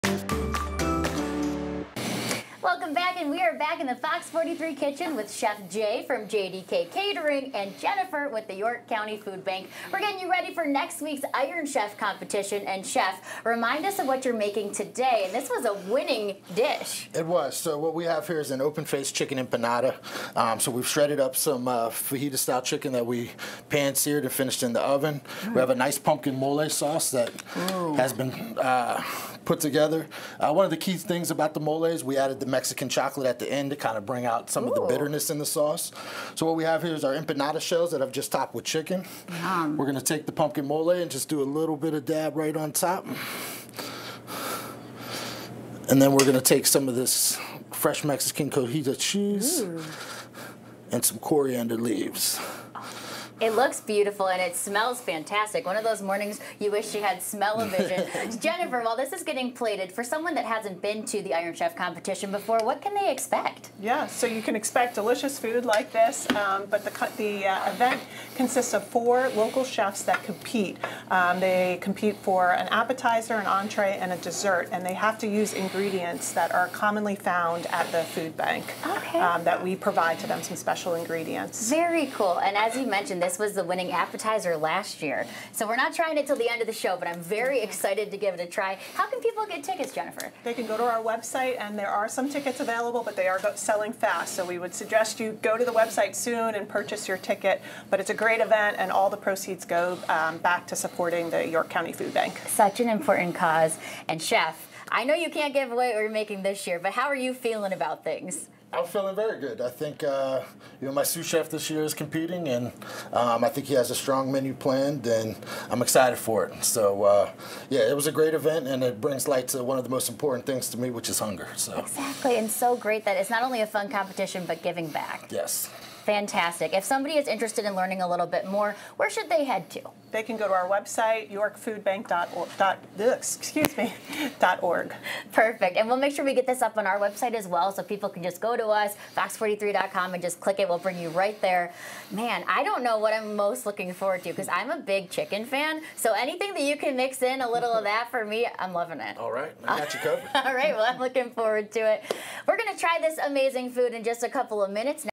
Chefs go head-to-head in effort to feed York County community
Four local chefs are locked in their kitchens, preparing ultra-secret menus to crush their competition.

Iron Chef takes place next Monday at Wyndridge Farm. Each cook will create an appetizer, entrée and dessert in hopes of wowing the judges and taking home the title of Iron Chef. Their menus must include ingredients often found at the York County Food Bank.

"So we require the chefs that are competing in the Iron Chef event to use—we pick out some common ingredients that are commonly found at the food bank," Jennifer Brillhart, president and CEO of the York County Food Bank, said. "The chefs help to elevate that and show, demonstrate, how you can use these simple ingredients and make them into what you're seeing today."